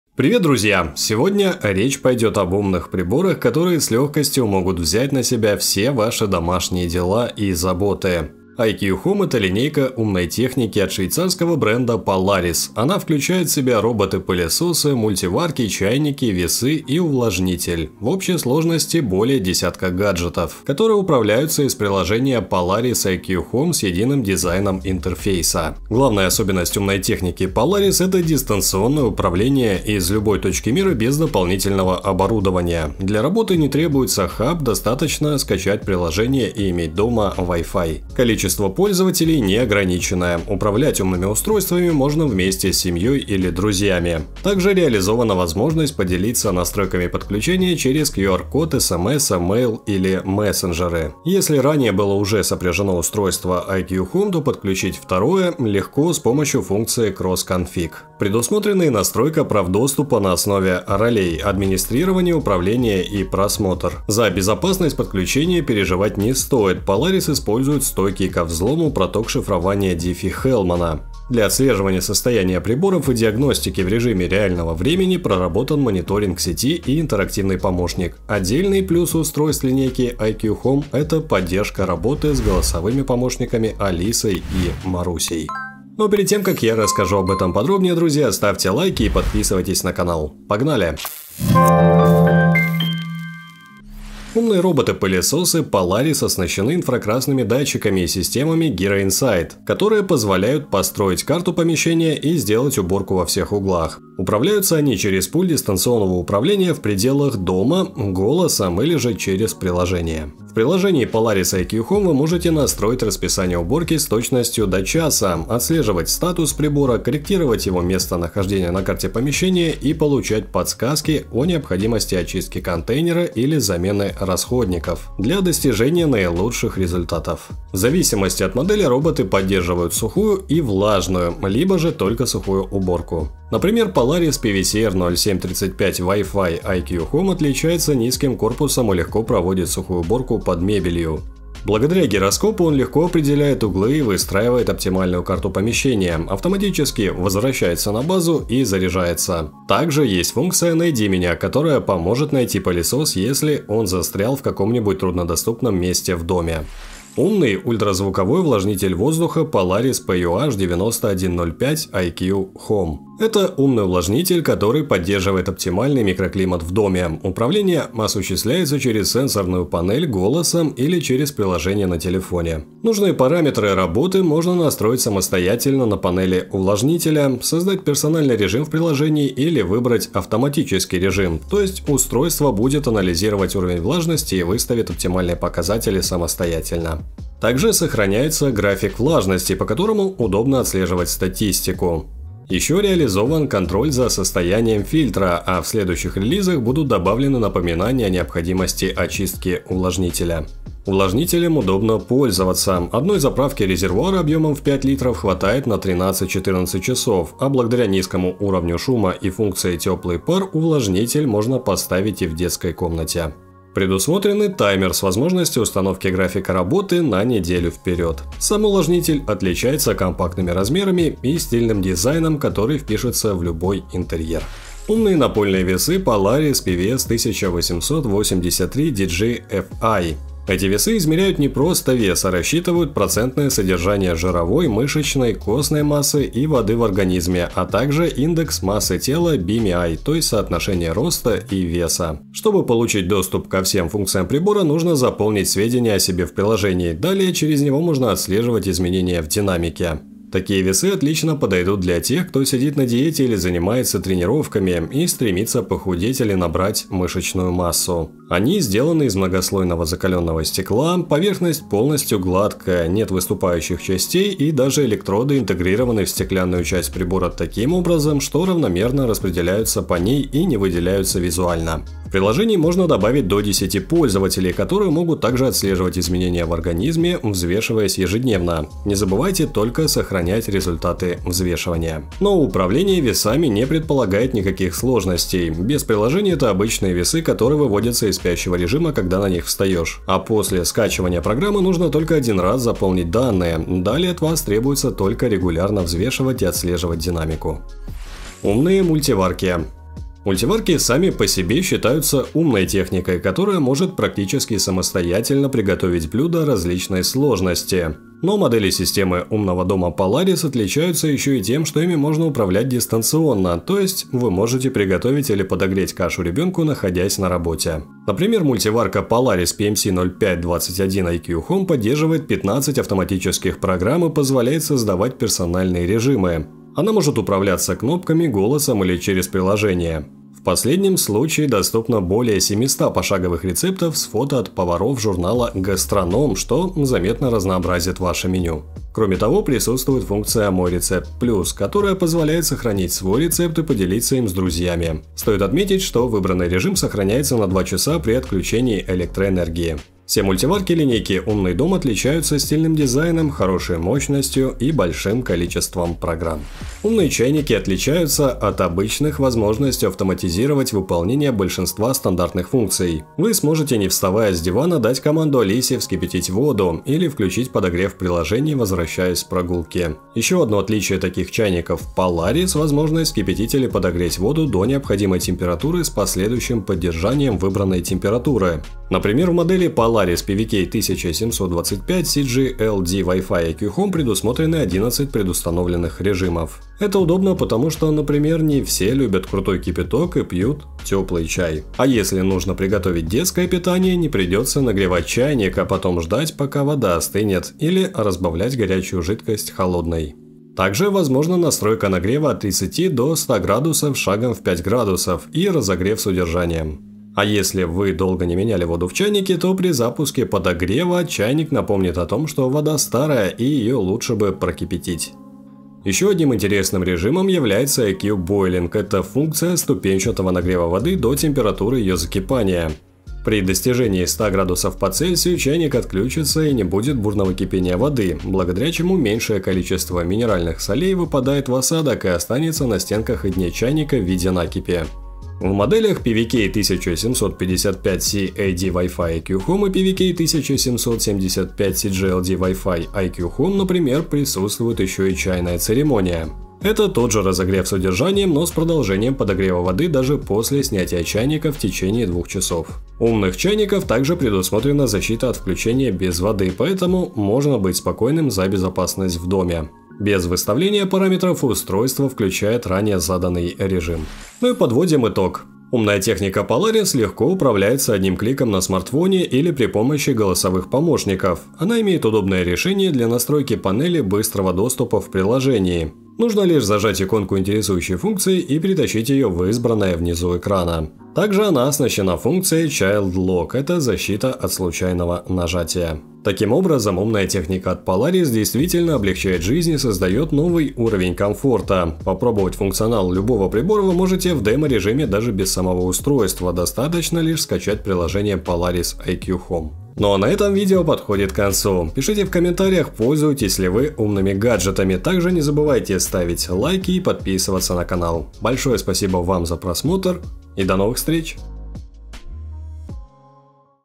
Умный дом. Что он может? Зачем он нужен? Что такое умный дом? Обзор умного дома Polaris
Система умный дом, зачем она нужна, что такое умный дом, как он может упростить вашу жизнь? Все это вы узнаете из нашего ролика.

Хочешь первым узнавать о новостях из мира техники и hi-tech?